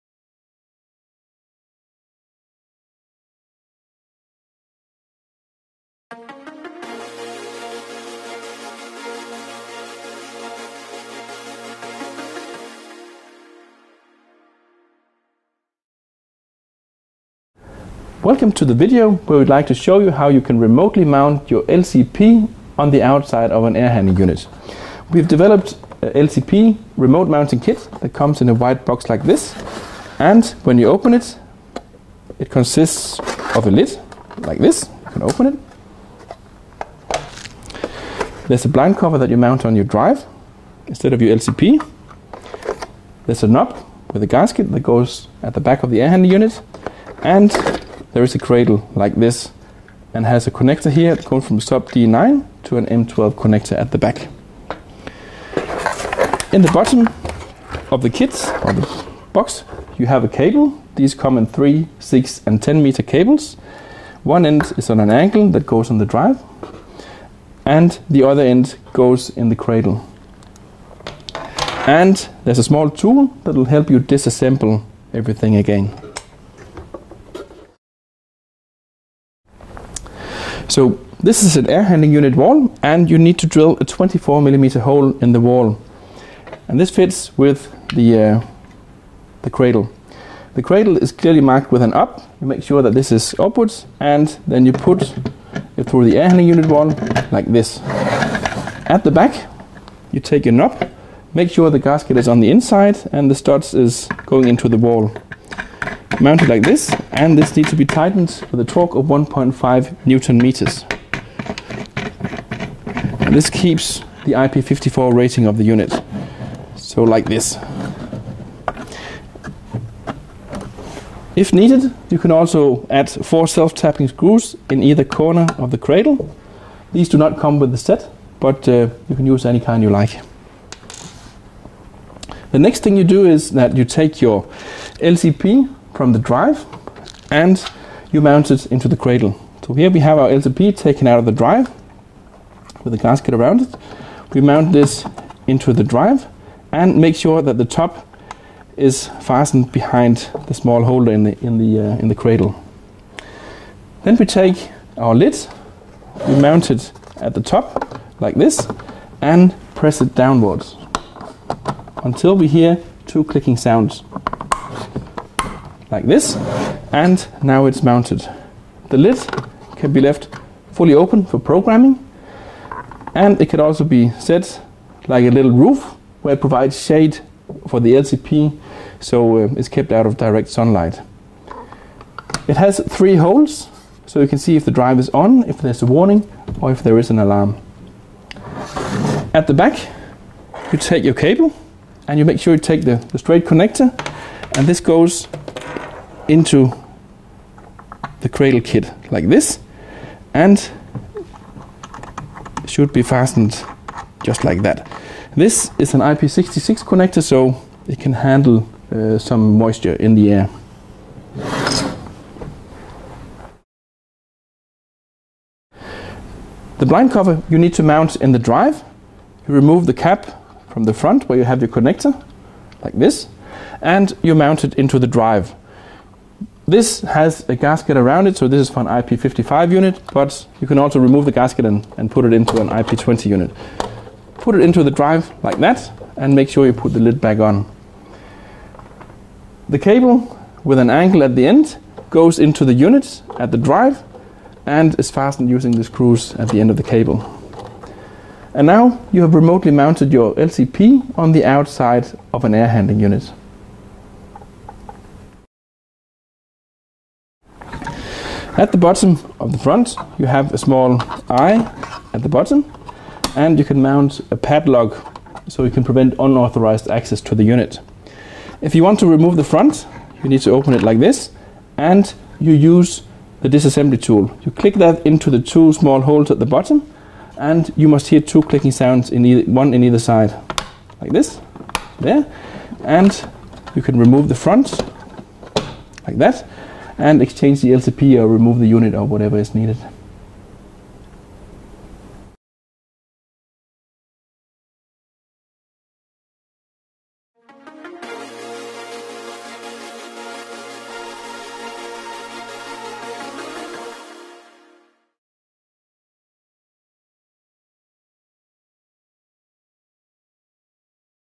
Danfoss Video of Remote LCP mounting kit for VLT® Inverter Drives
CSE Distributors supplies the Danfoss VLT® range of drives, this is a Danfoss produced video of their remote LCP mounting kit.  Contact our Wooburn Green or Eastleigh branch for more information.